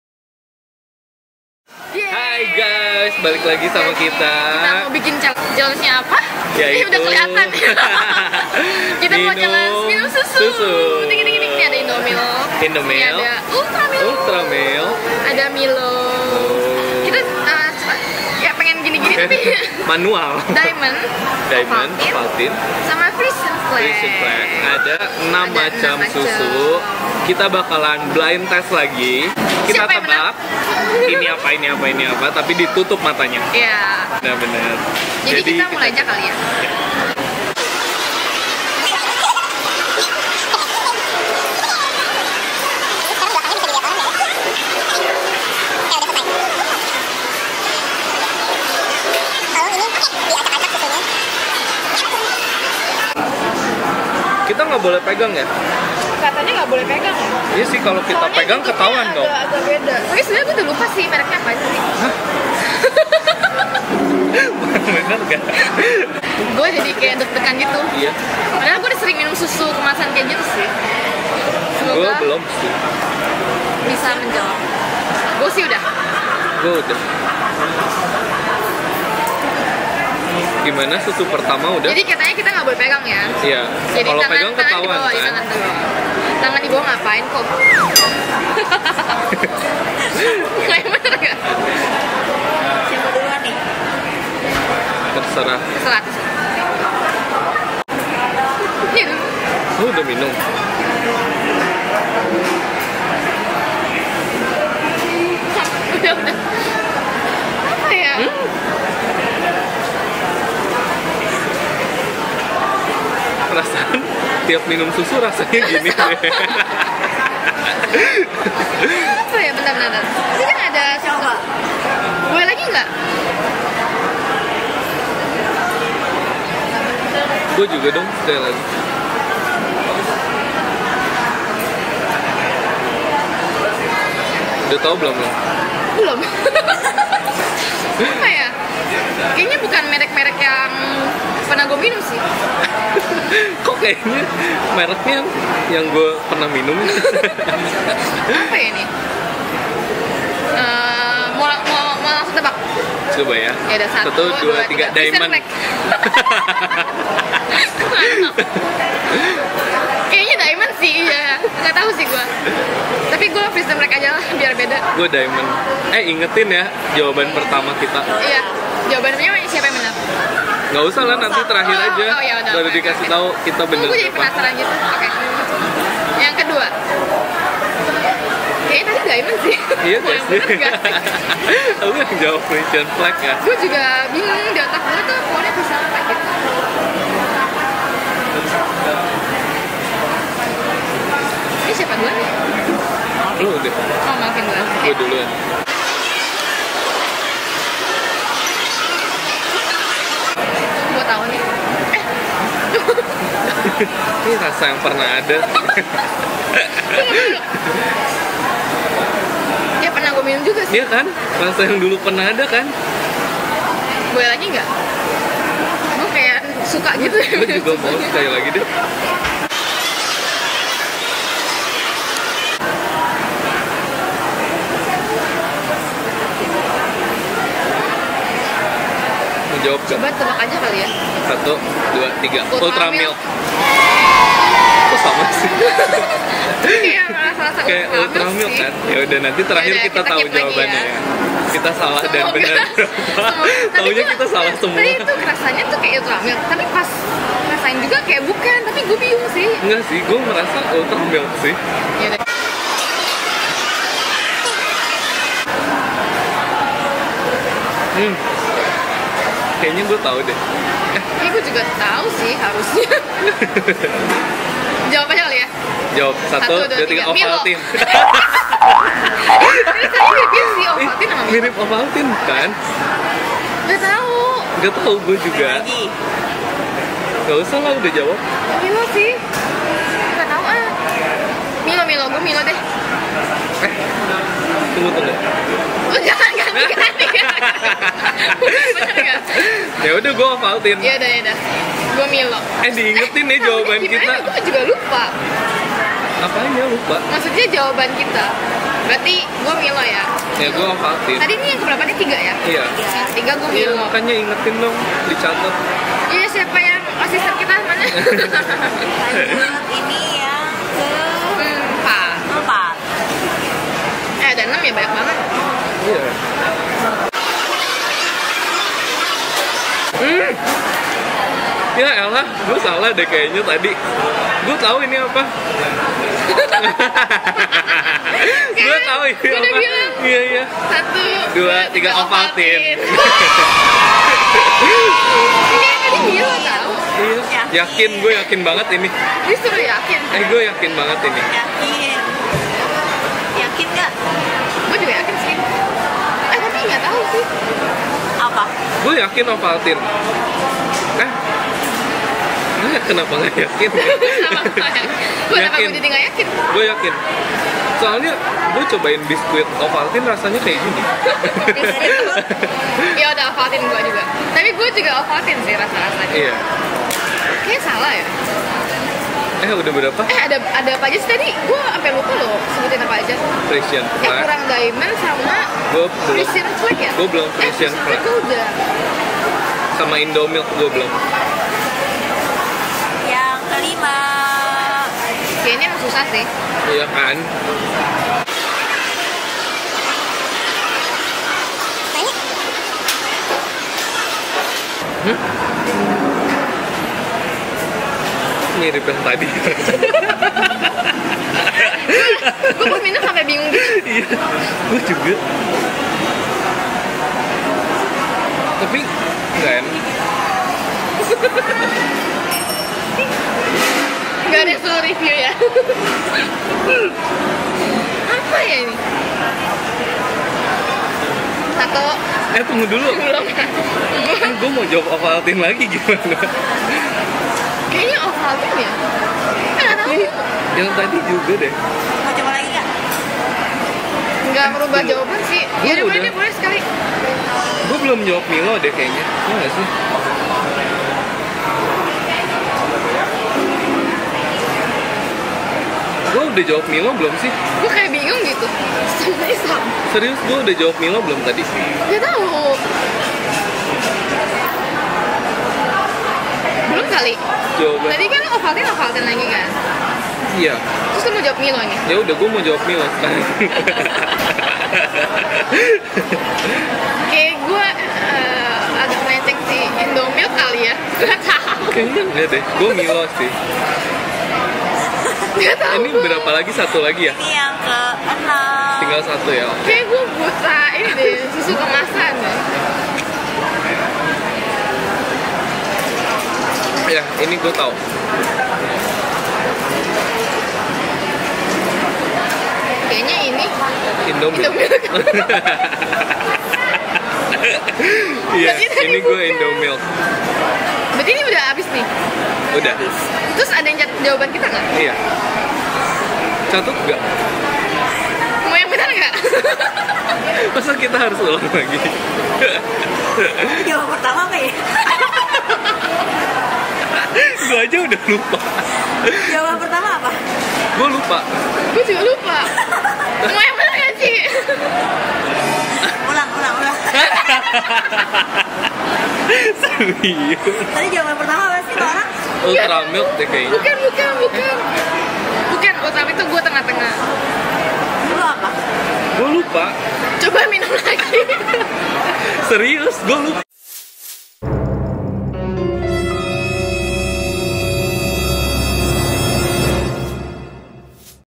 BLIND TEST SUSU KEMASAN
Kali ini kita bikin challenge BLIND TEST Susu Kemasan Rasa Cokelat
Hayo menurut kalian siapa yang menang?
Many thanks to Ci Vina dan Ko Apho dari @shecreatebelly yang udah bantu jadi Host dan Juri kita plus Ko Hara @storeateller yang sudah jadi supporter kita